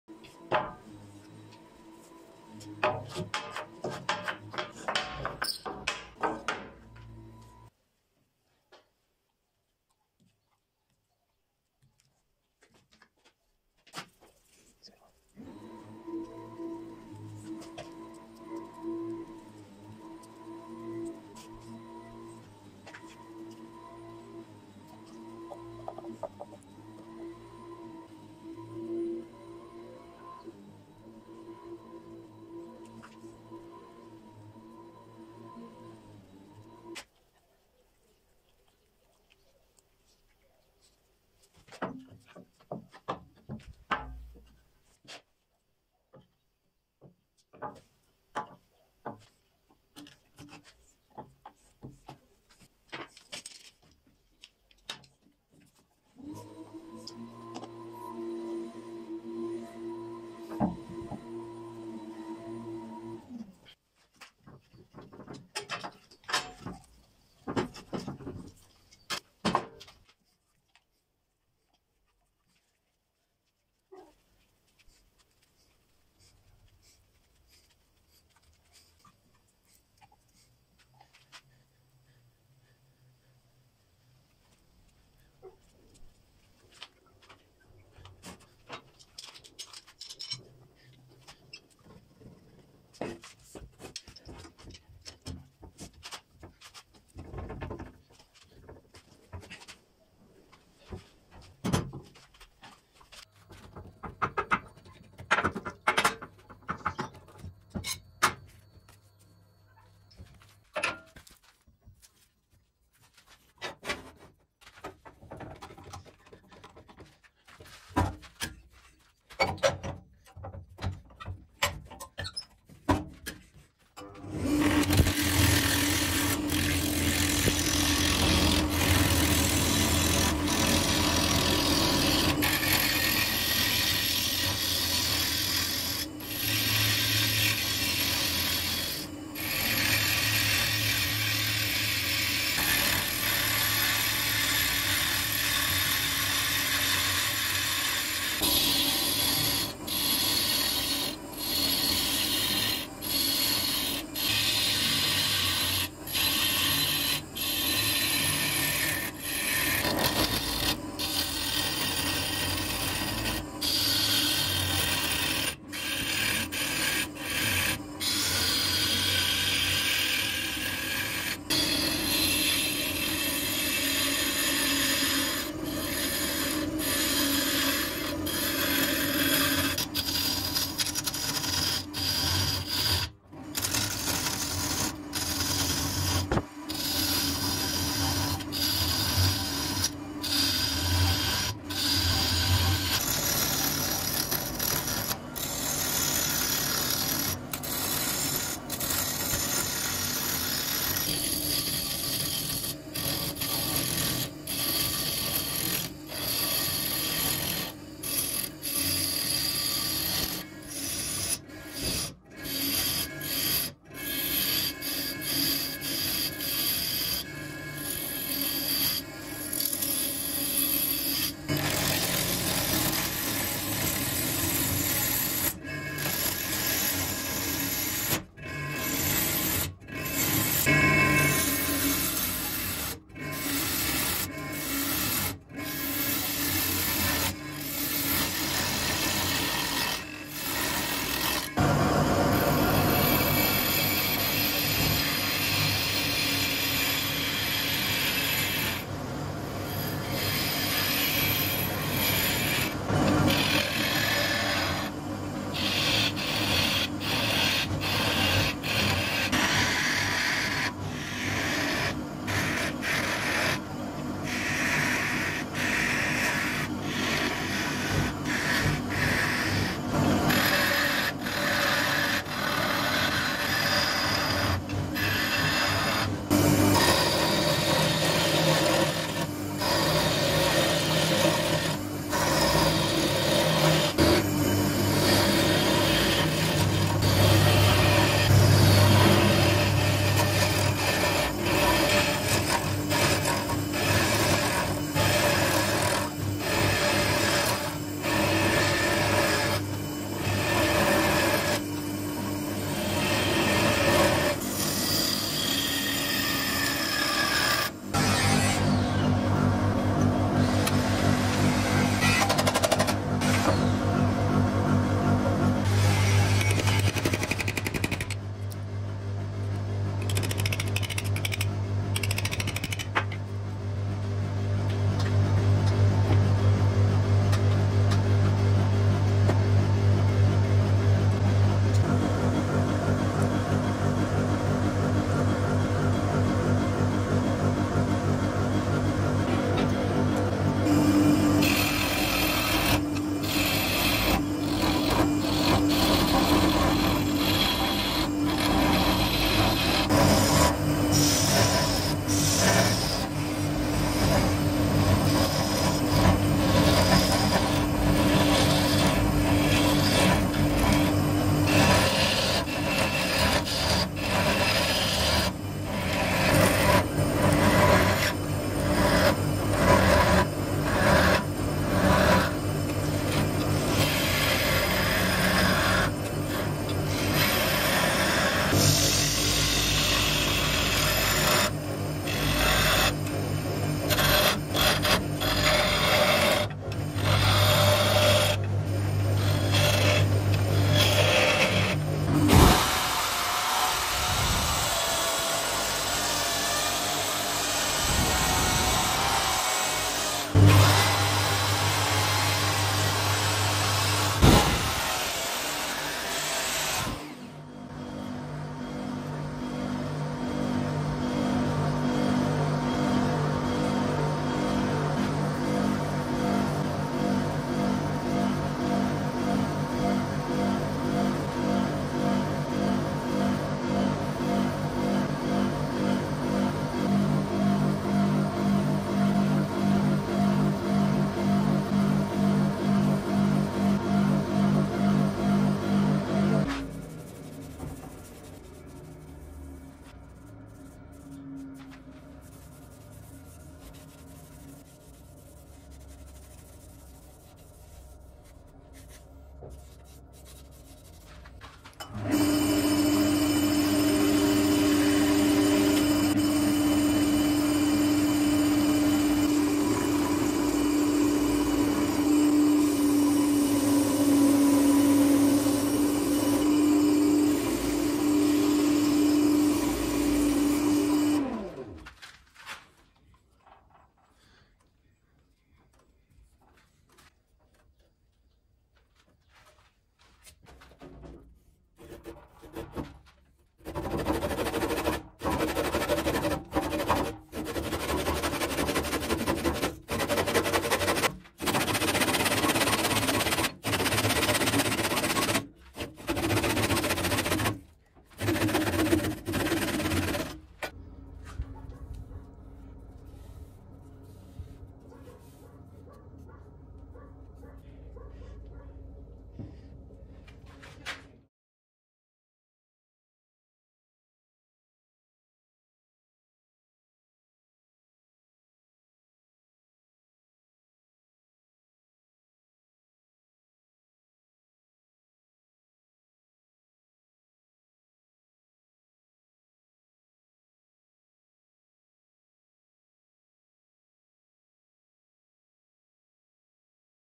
Woodturning - Plank Vase
Turning a stack of apple tree stems into an original vase.

If you find my Woodturning videos entertaining don’t forget to share them with your likeminded friends.